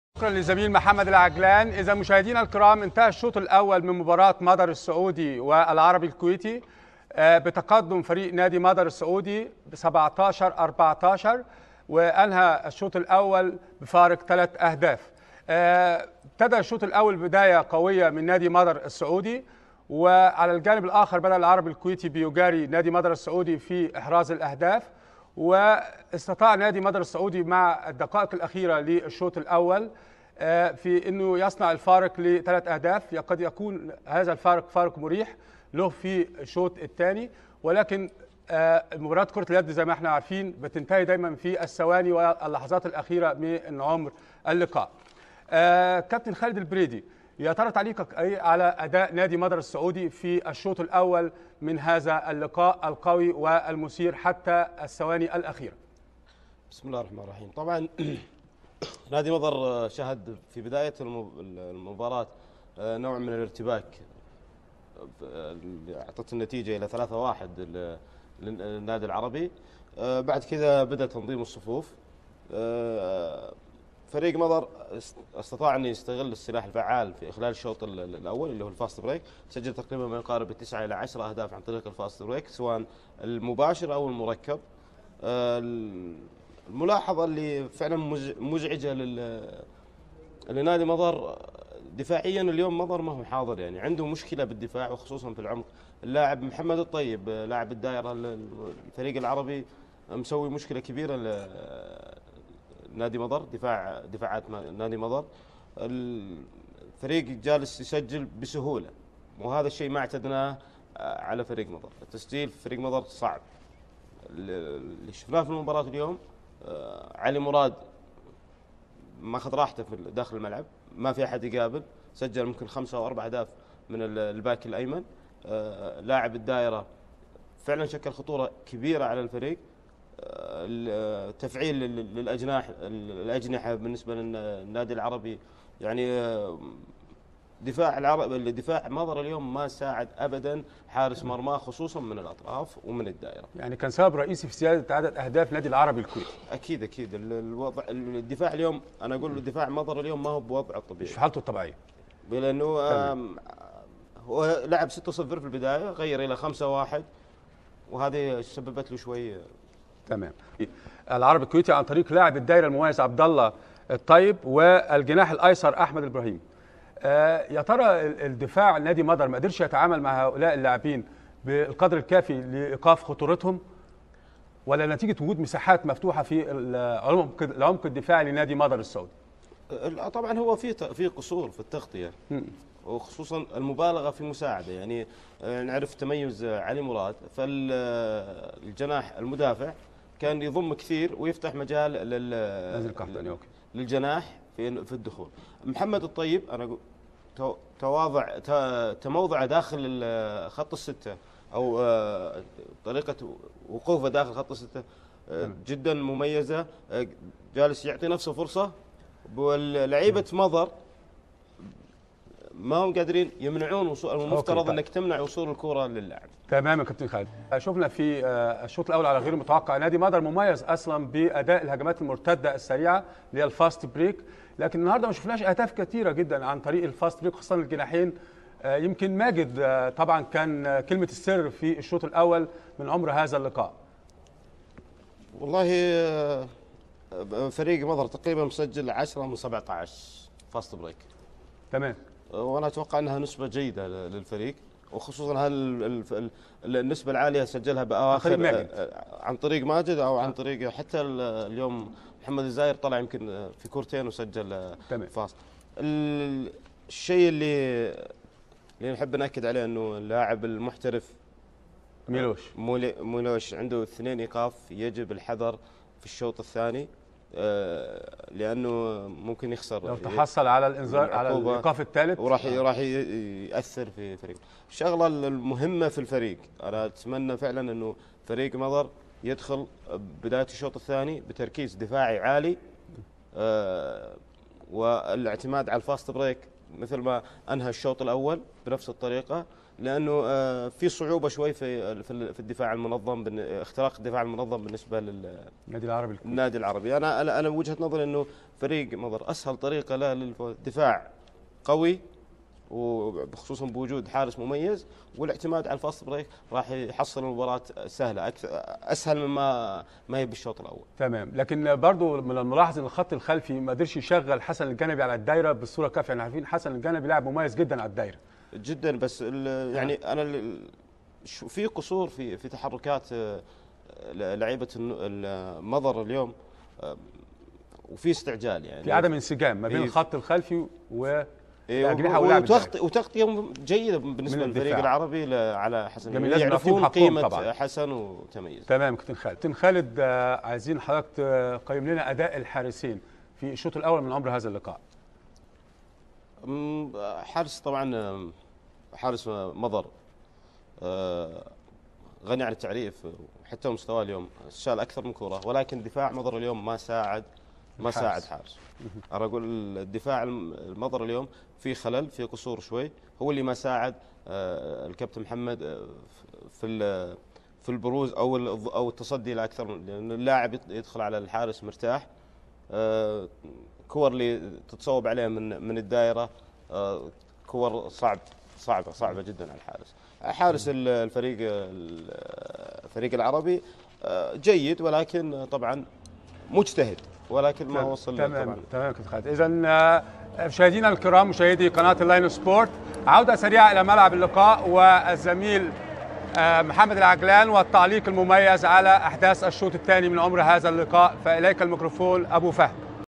خالد البريدي Khaled Albredy بين شوطي مباراة مضر و العربي ضمن بطولة الاندية الاسيوية 2013
الاستديو التحليلي للكابتن خالد البريدي (Khaled Albredy) قبل مباراة مضر و العربي ضمن  بطولة الاندية الاسيوية 2013 في الدورم الاول حيث النتهت المبارة بقوز نادي مضر 29 -26